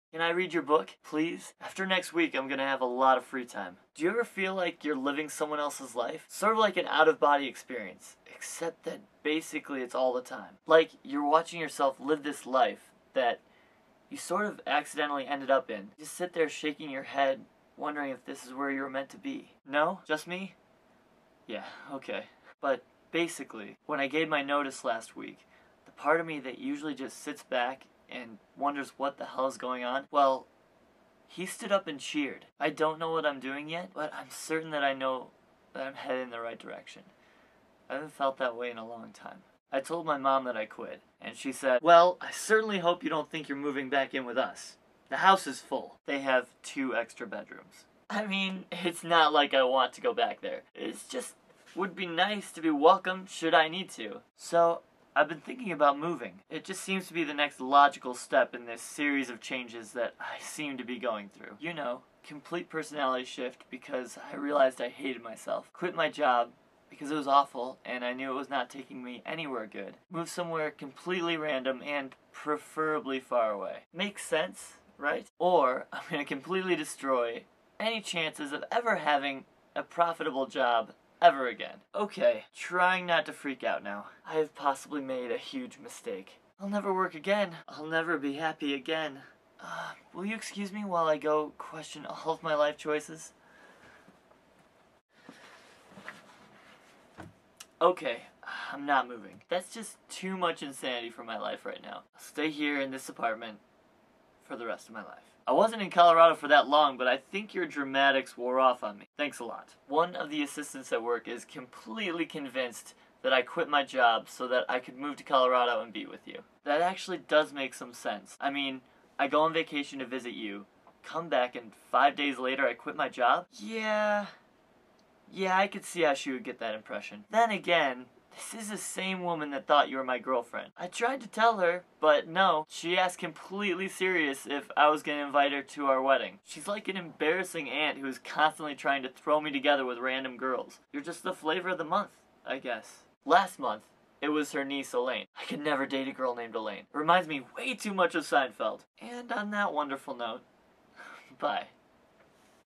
Ep. 26: Question Your Life Choices
I have made a terrible mistake and am freaking out!
Starring
Ian Bailey- Luke Loomis
Waverly Wilson- Anna Milvain
Evangeline Wilson- Karina Milvain
Myca- Michele Abplanalp
Amanda- Michelle Tebedo
Josh- TBA

Created by Anna Milvain
Written by Anna Milvain with Karina Milvain
Cinematography by Abby Chu
Edited by Abby Chu